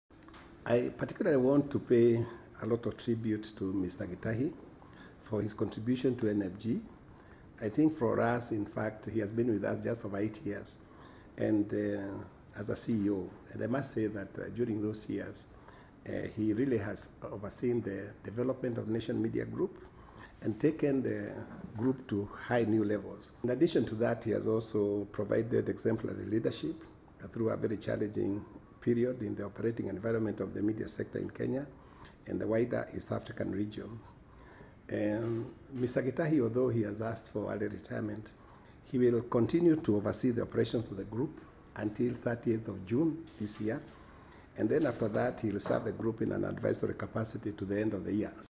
Nation Media Group gets new CEO
The Nation Media Group Board has announced the appointment of a new Group Managing Director and Chief Executive Officer, effective from 1st July 2015.
Joseph Muganda succeeds Linus Gitahi, who has requested early retirement to pursue personal interests. Muganda is a highly reputed senior executive with vast experience in general management and marketing. He joins NMG from Kenya Breweries Limited where he is currently the Managing Director.Announcing the changes, the NMG Board Chairman Wilfred Kiboro paid tribute to Mr. Gitahi for his contribution to NMG during the eight years he served as CEO. Gitahi will continue to oversee the operations of the group up to 30th June and serve the Group in an advisory capacity to the end of the year.